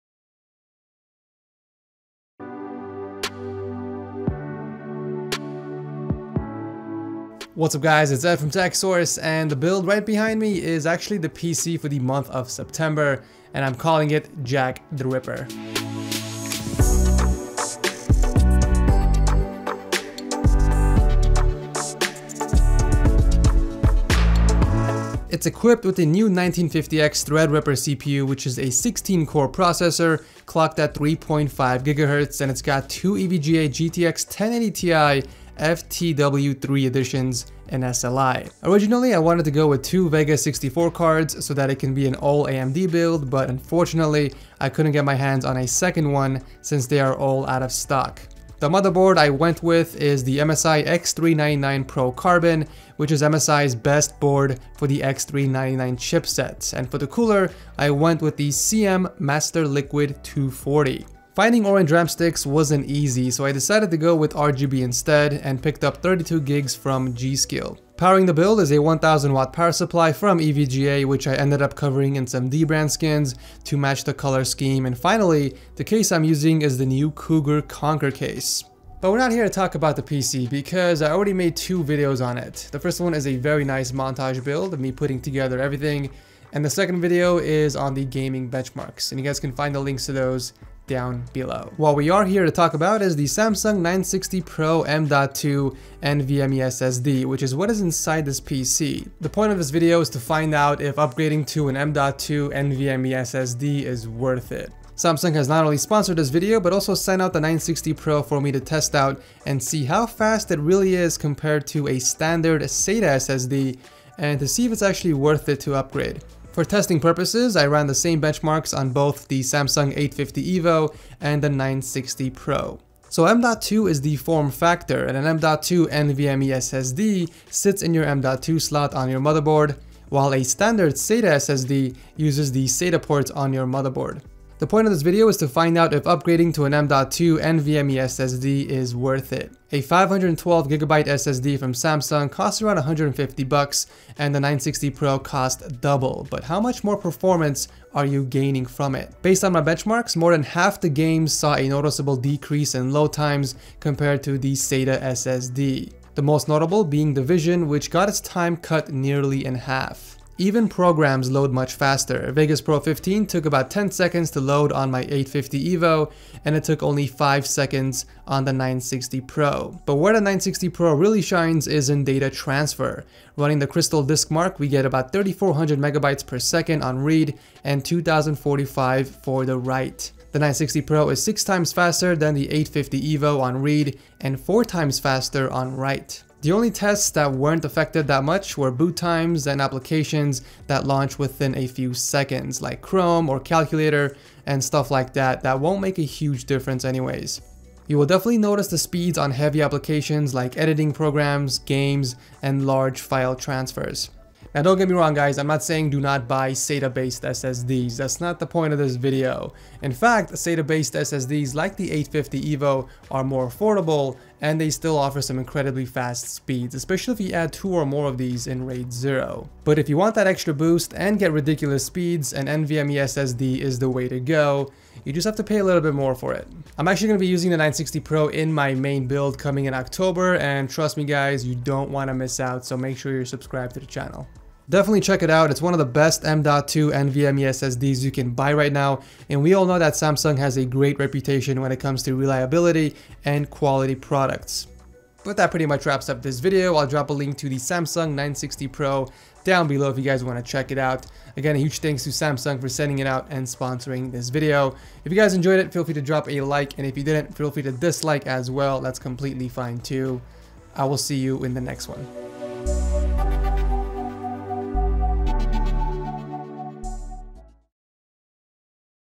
Is an NVMe SSD Worth it? | Samsung 960 PRO
Should you upgrade your current storage to an M.2 NVME SSD?

►Time Lapse Build: *Coming Soon*
►Benchmark Video: *Coming Soon*